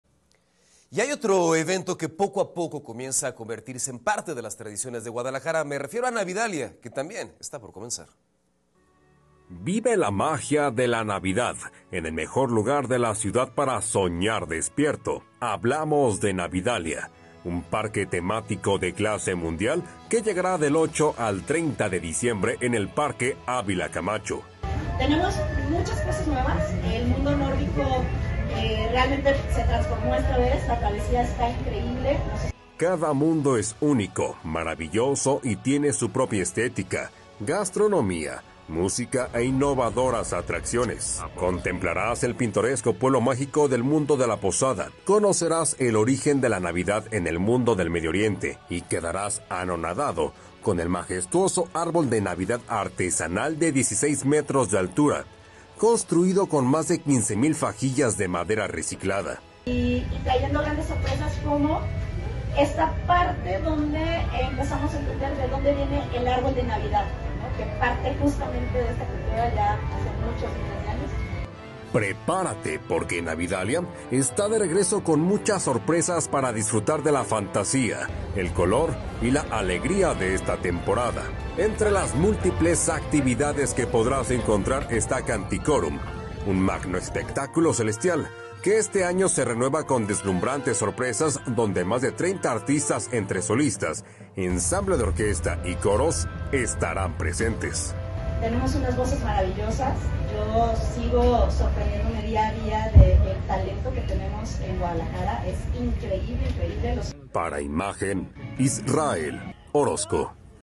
¡Por fin llegó 'Navidalia' a Guadalajara! | Imagen GDL con Ricardo Camarena
Este ocho de diciembre llega ‘Navidalia’; parque temático que cada año llega a Guadalajara, esta ocasión con más sorpresas, ¡no te lo puedes perder!